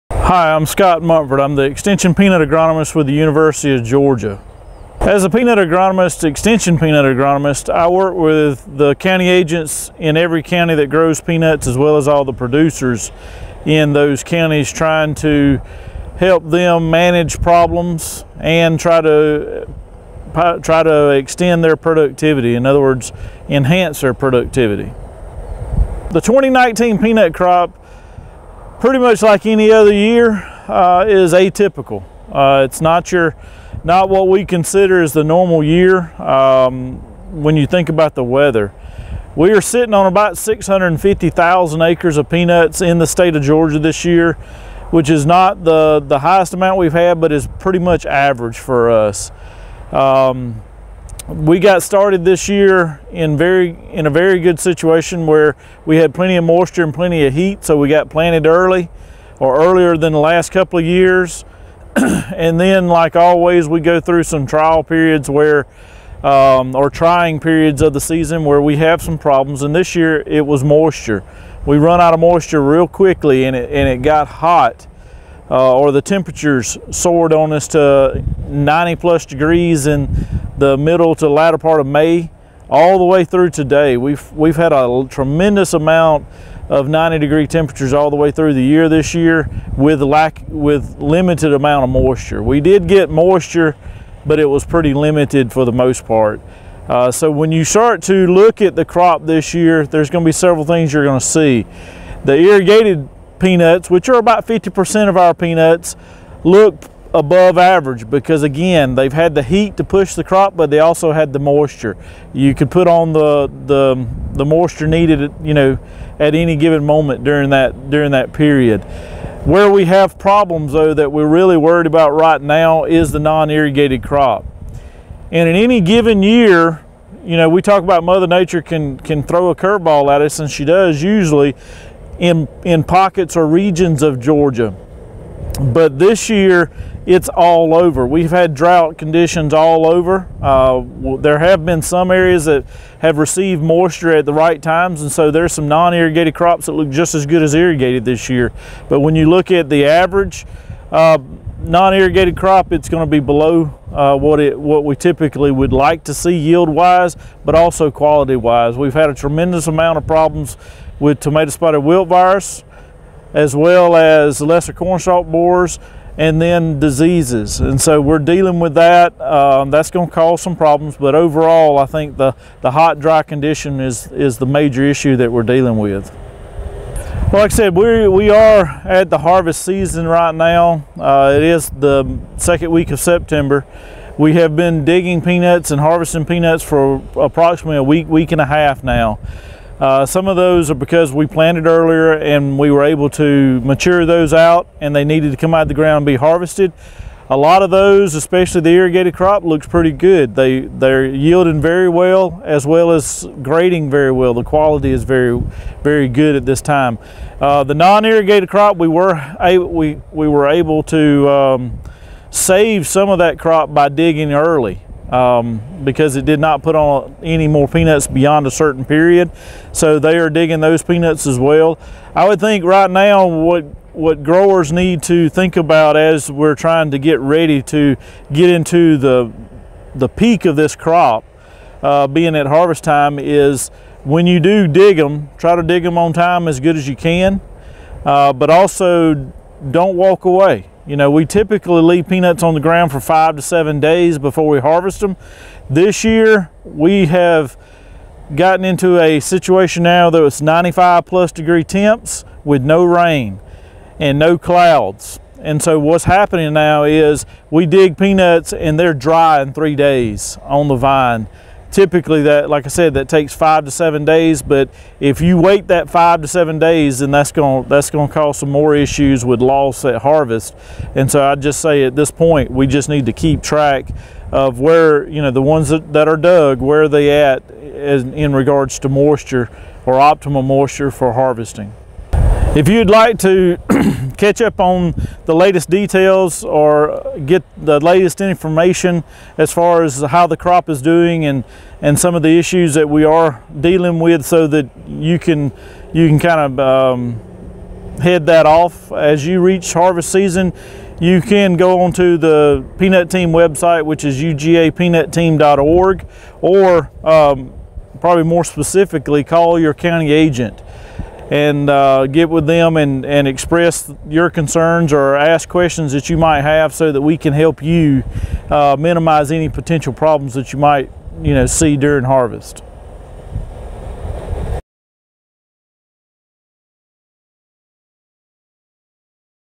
2019 GA Peanut Tour - Scott Monfort
The Georgia Peanut Tour kicked off with a Hot Topics Seminar on Tuesday, Sept. 17 at Lake Blackshear Golf Resort in Cordele. During the seminar Scott, Monfort, University of Georgia Extension Peanut Agronomist. According to Monfort the 2019 peanut crop pretty much the same as most years but the weather is not what you would consider a normal year. Georgia growers planted approximately 650,000 acres of peanuts in 2019.